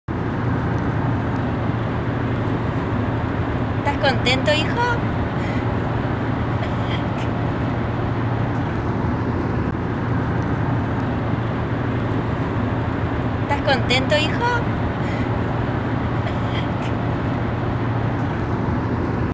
La alegría de Renzo en su viaje a China con la fe y la esperanza de recuperar la visión
El pequeño y su familia parten a Beijing donde iniciarán el tratamiento para poder ver. En diálogo con Elonce TV, sus padres se mostraron muy agradecidos con la ayuda de la gente que les permitió juntar el dinero. "Vamos con mucha fe".